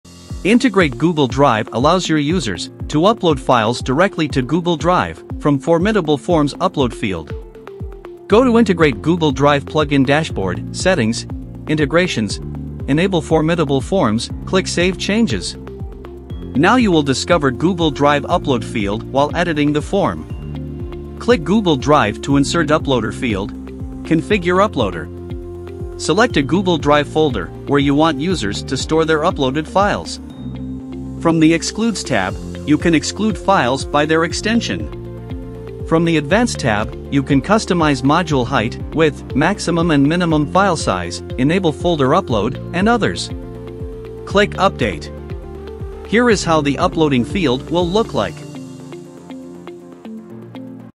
How to Upload Google Drive Files from Formidable Forms Upload Field
This feature allows you and your users to upload your files directly to Google Drive from your Formidable Forms upload field.